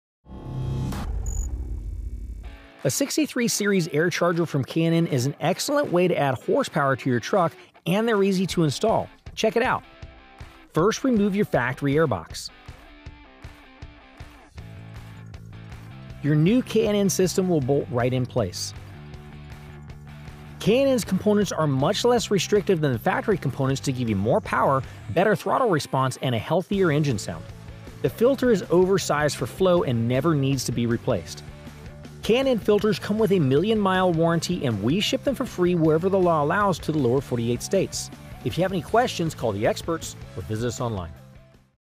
Mods in a Minute - K&N 63 Series AirCharger High Performance Air Intakes
Add horsepower, torque and ensure a better fuel economy all in one package, with the K&N 63 Series AirCharger High Performance Air Intake. K&N 63 Series air intakes are guaranteed to add horsepower and acceleration to most vehicles out there. These tubes allow large amounts of cool air to flow unrestricted, ensuring better combustion, adding horsepower and even bettering your fuel economy.